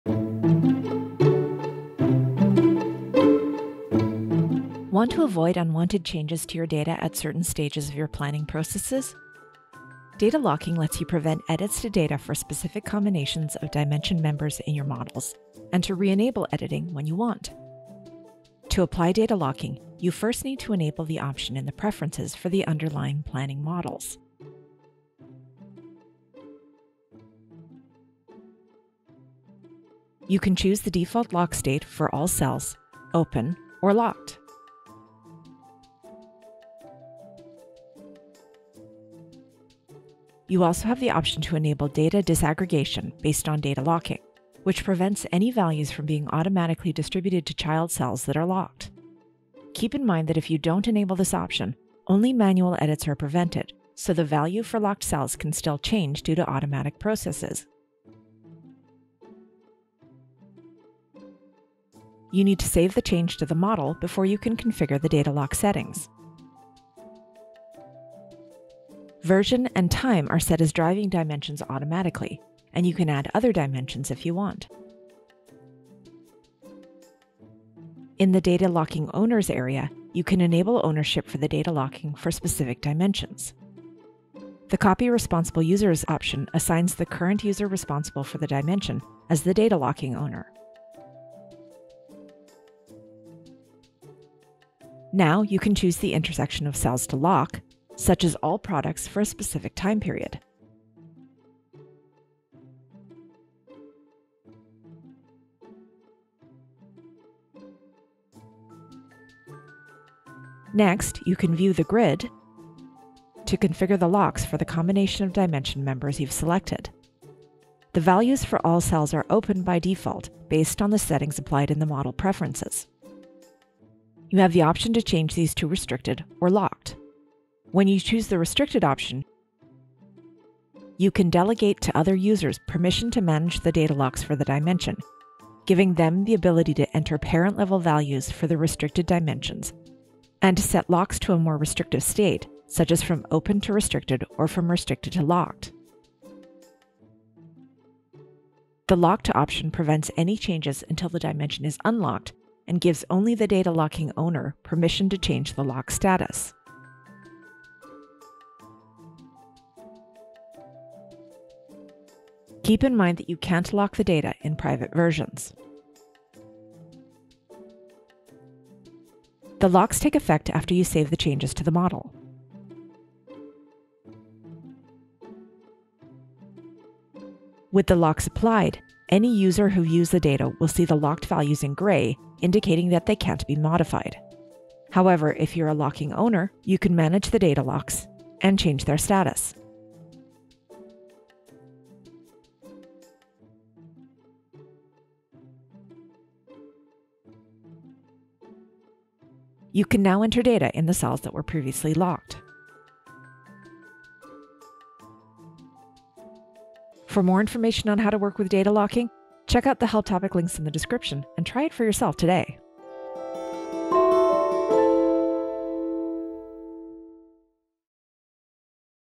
How to Plan Using Data Locking: SAP Analytics Cloud
In this video, you will apply data locking on the dimensions of a planning model to prevent unwanted changes during the planning process.

Chapters:
0:00 Enable data locking in the model
0:40 About data disaggregation based on data locking
1:07 Configure data locks
1:50 About restricted and locked status
2:56 Manage data locks in a story

Recommended Help Topics:
• Configuring Data Locking: